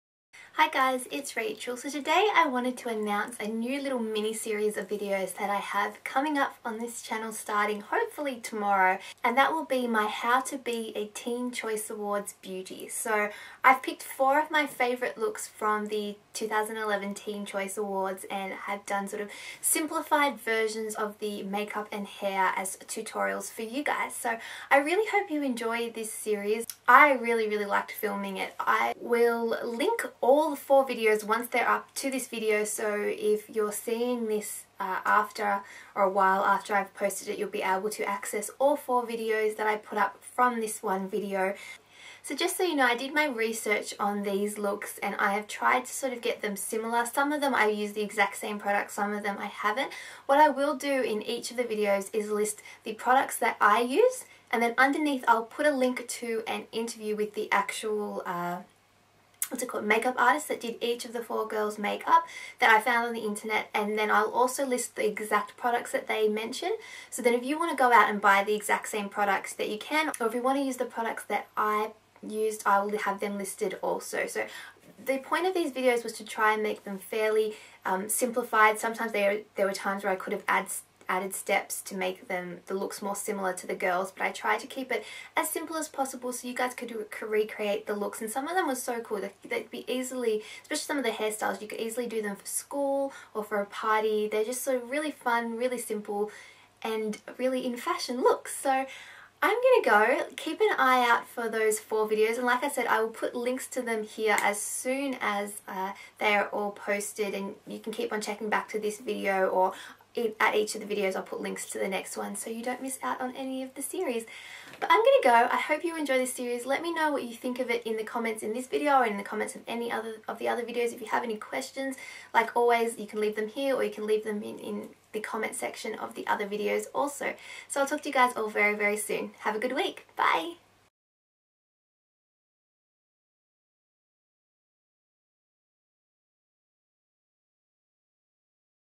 How to be a 2011 Teen Choice Awards Beauty: Series introduction
PLEASE OPEN FOR MORE INFO

Hi Everyone,

This is just a quick video announcing a little series of celebrity inspired tutorials I will be uploading this week all surrounding some of the hair and makeup styles at the 2011 Teen Choice Awards.

xo xo Rach